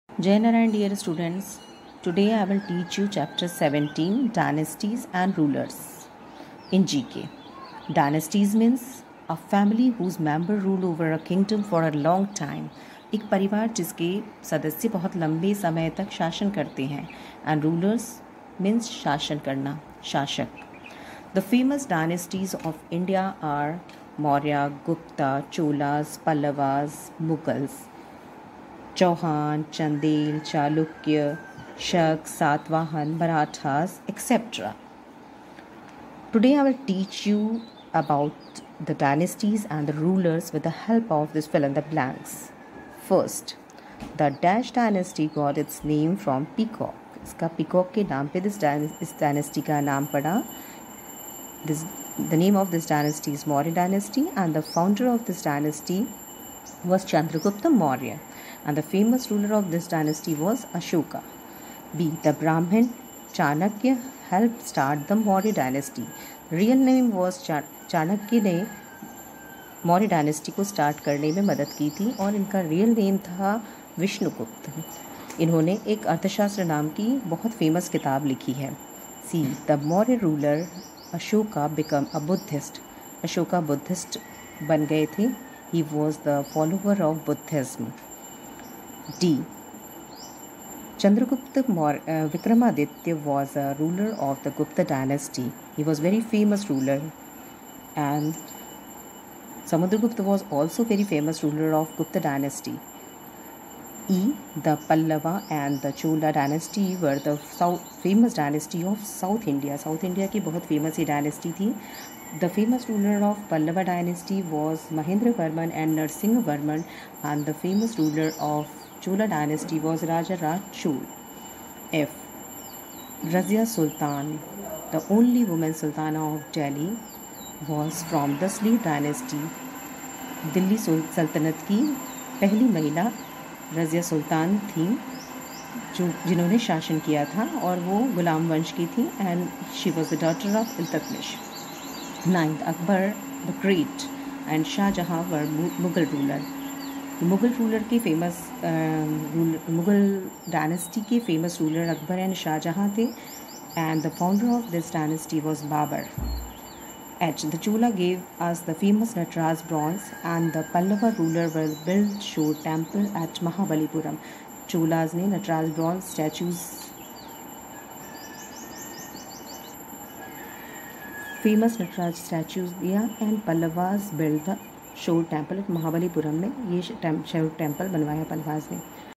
Class : 5 ABC subject : G.K.
Topic : famous dynasty and ruler of India
Prepared by : Anamika Mishra